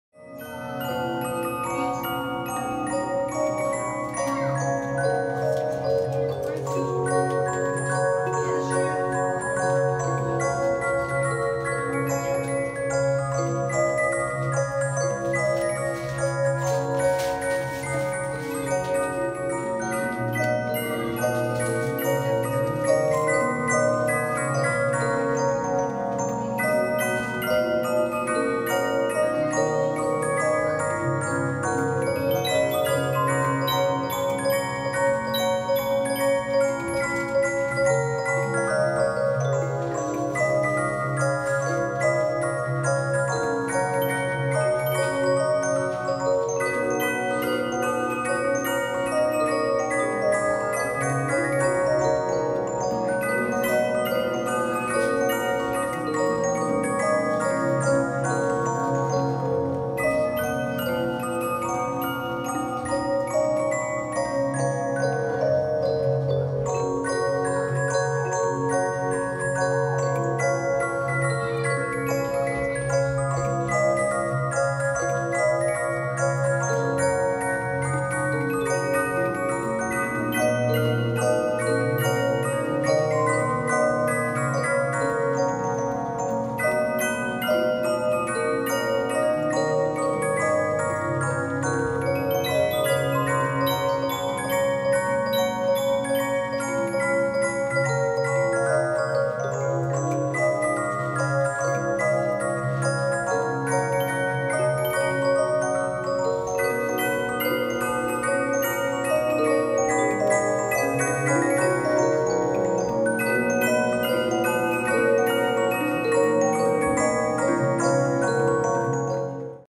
P Nut Store Tour 2018
Store Tour of Western Nut Factory Store in downtown Salt Lake City 2018. P-Nut takes us around our store so you can see all of the amazing gift ideas we have. Musical background provided by our very own Porter Music Box - come by and see it for yourself.